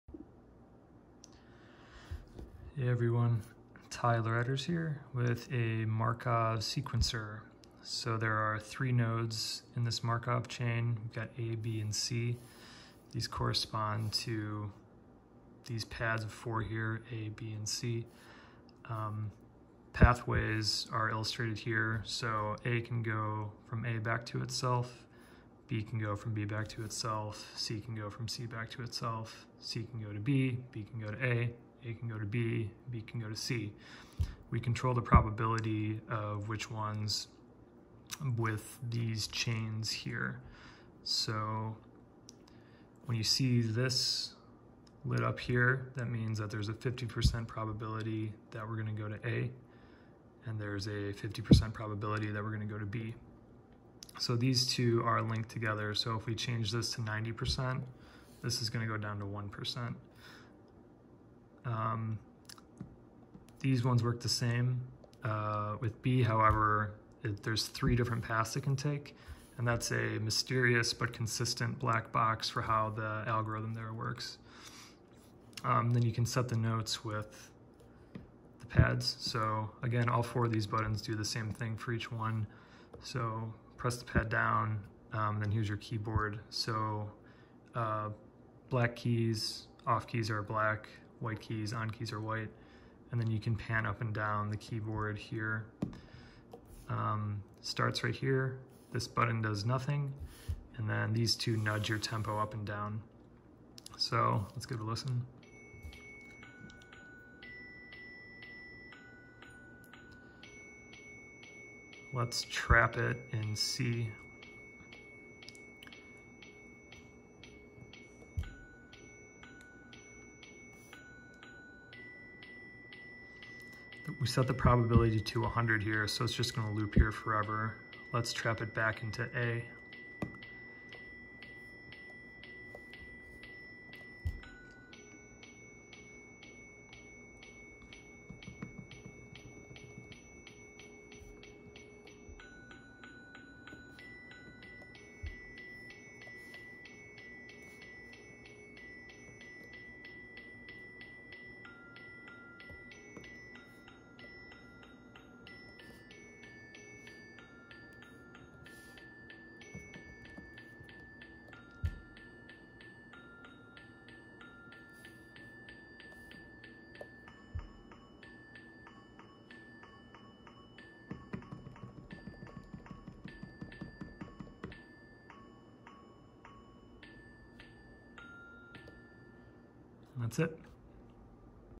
Markov Music Demo [monome grid + Max]
Demo of a monome grid patch written in Max. The patch uses Markov chains to generate music.